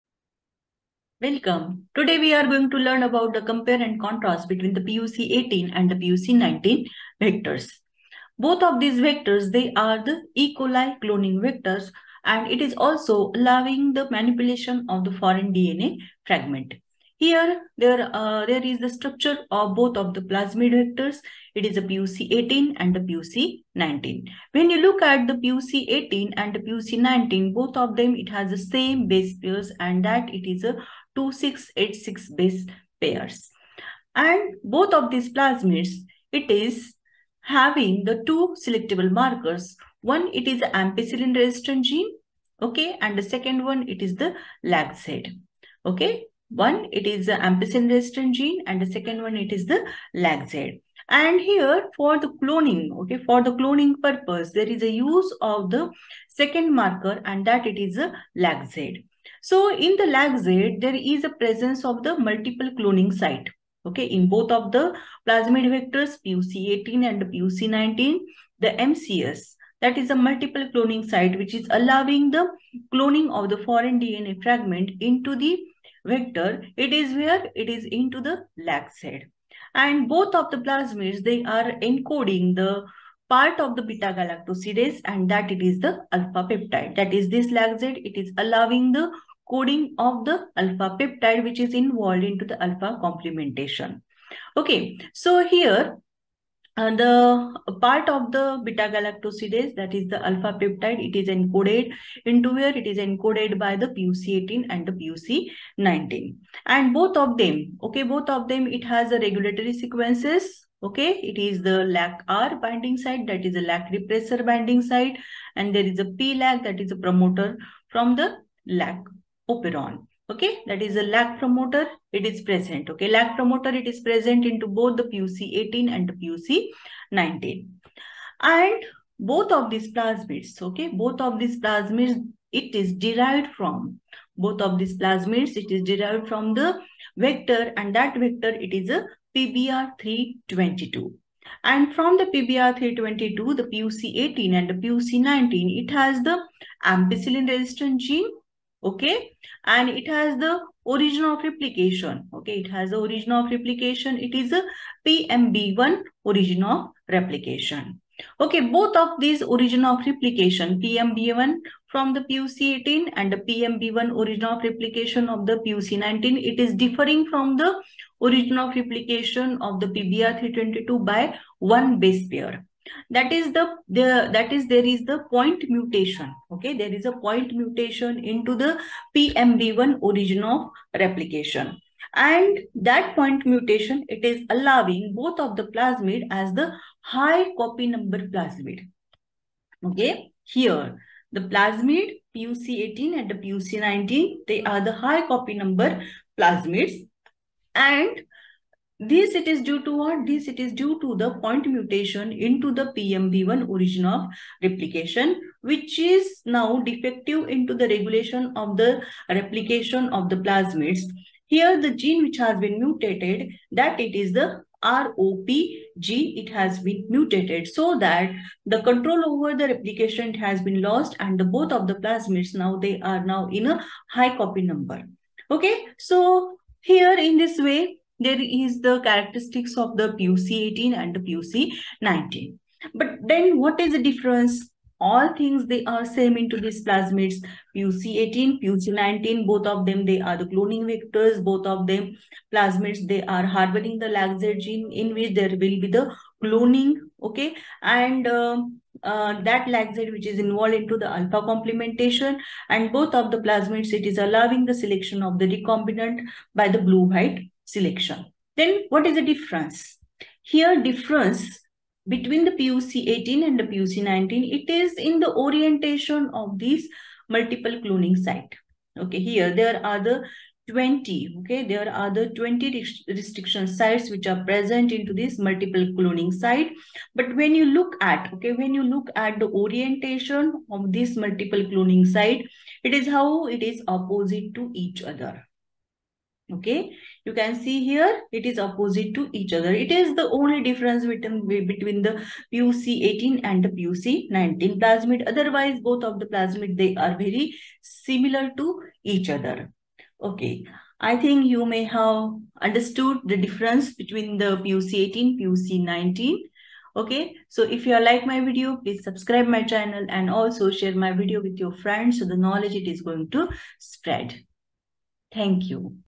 Difference between pUC18 and pUC19/Compare and Contrast:pUC18 and pUC19/high copy no. plasmid vector
The discussion is regarding similarities and dissimilarities between pUC18 and pUC19 vectors. In detail focuses on characteristics of pUC18 and pUC19 vectors.